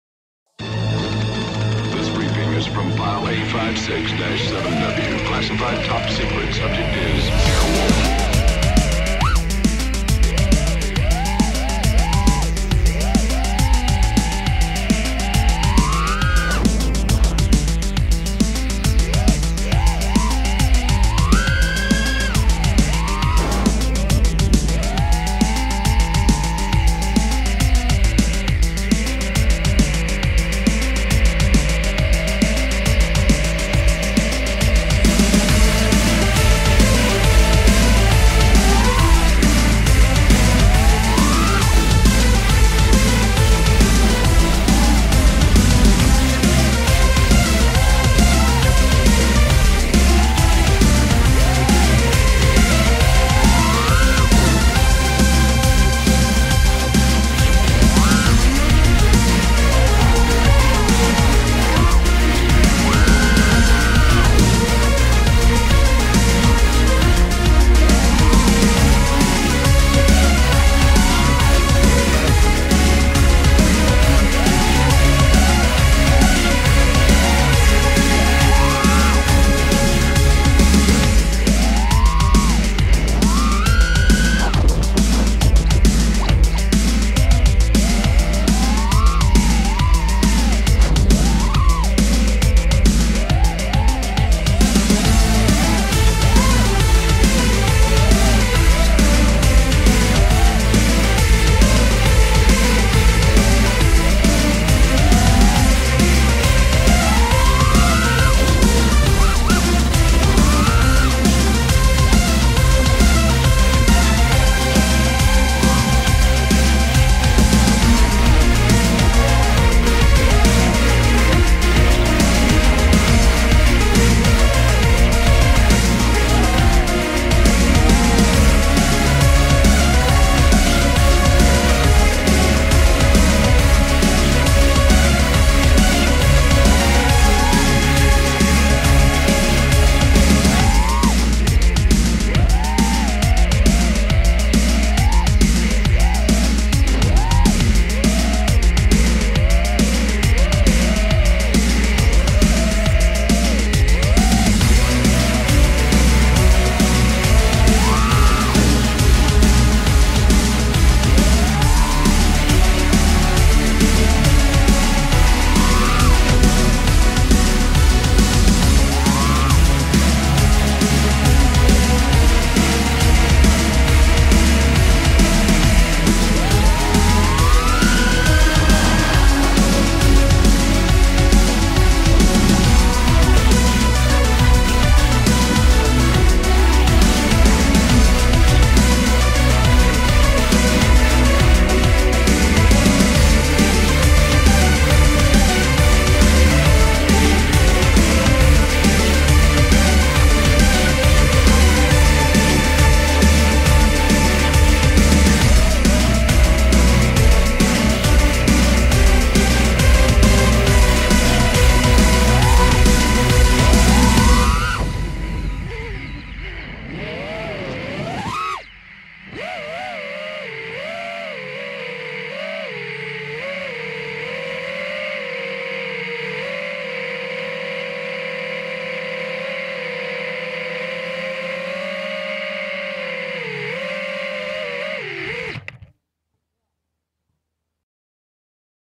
one pack one song rip uncut
went to the town park and ripped a 5s pack before total dark. for some reason i had this old ass tv show stuck in my head THANKS TO  @CHUCKIT!